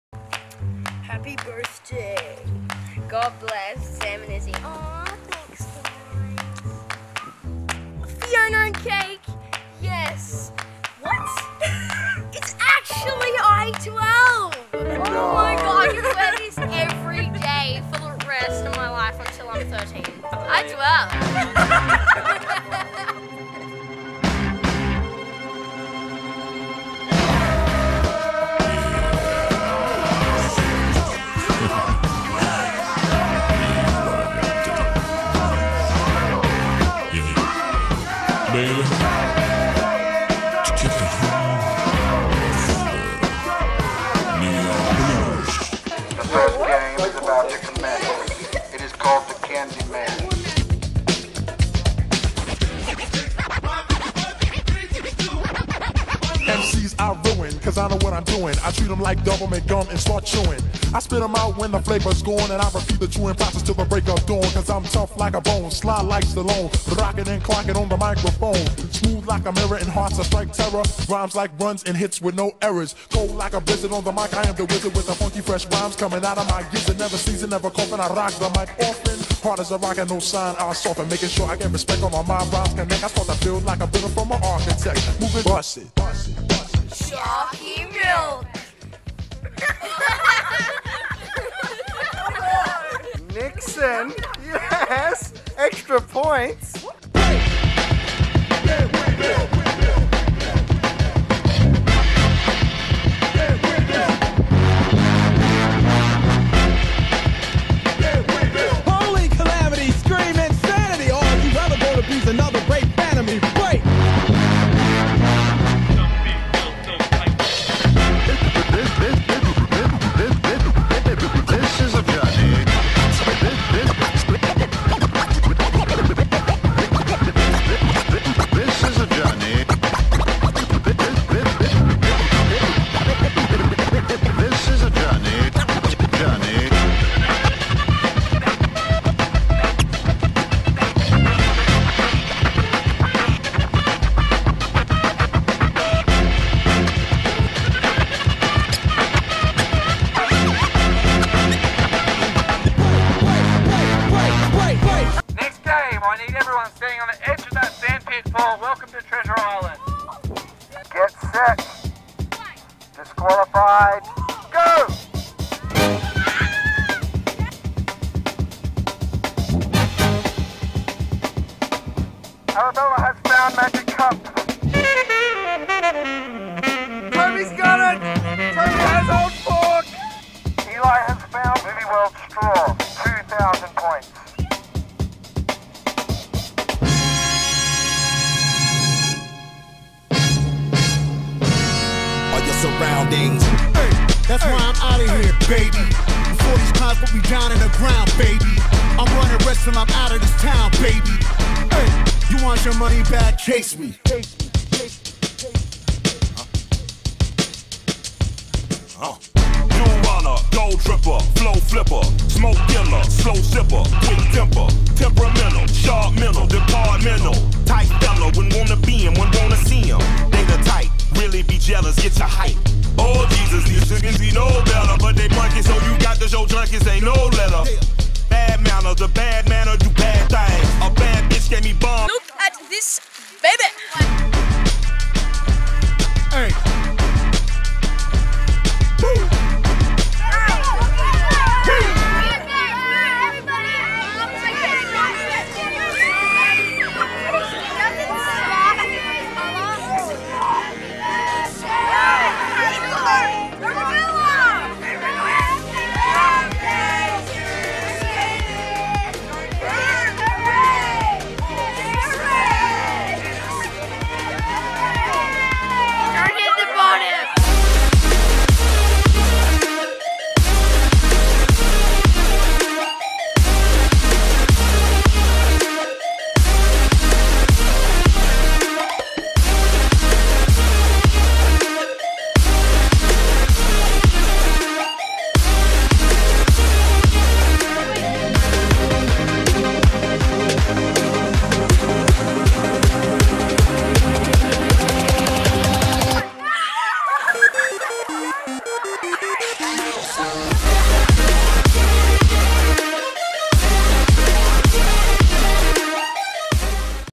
Winton's i12 Rubik's Cube Party
BDP 2017. feat my friends, wicked cool games and a ridiculous Rubik's Cube Cake. Thanks dad for organizing all this. Love.